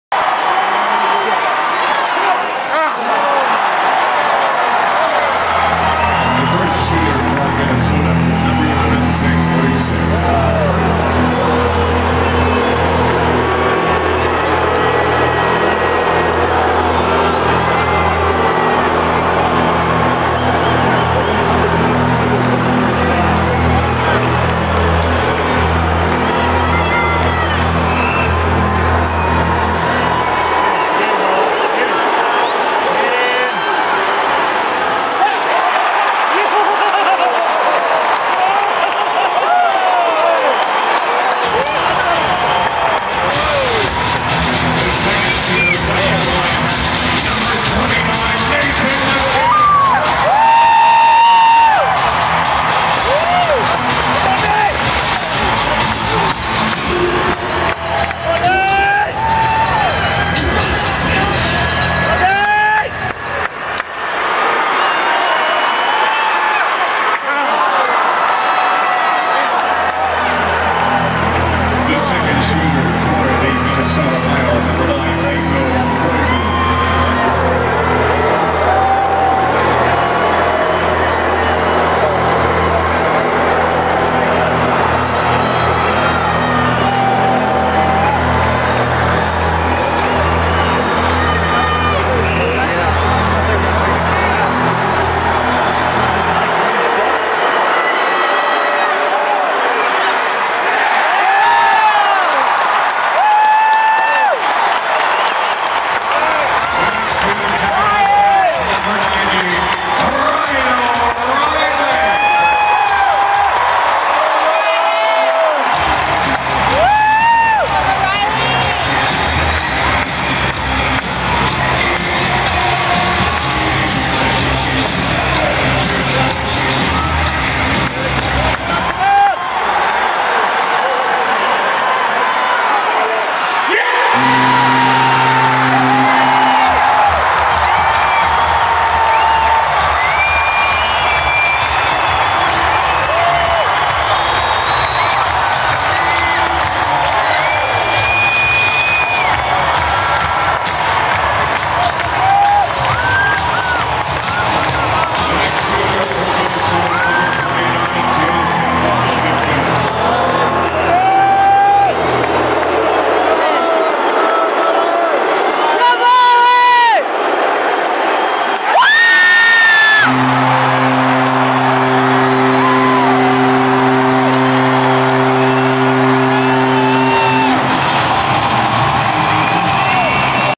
Colorado Avalanche
The Colorado Avalanche take on the Minnesota Wilds November 30. Colorado takes the lead with a goal from Ryan O'Reily in the shootout.